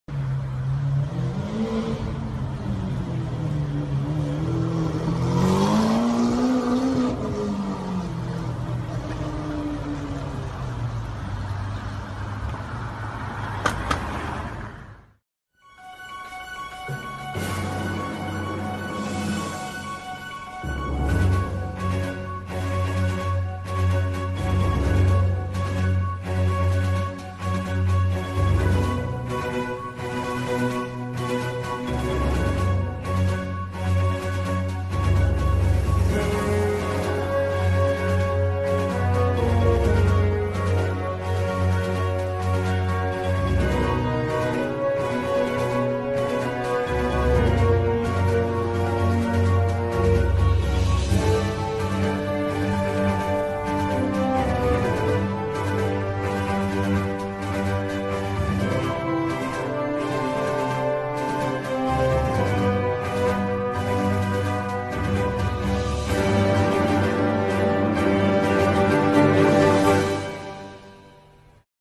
Full carbon Mansory Lamborghini ''Carbonado'' brings chaos in London!!! BODYGUARDS DRIVING MANSORY!!
The famous James Stunt driving his car in London with dons of bodyguards driving in Bentlys and Rolls royces. All Mansory!!!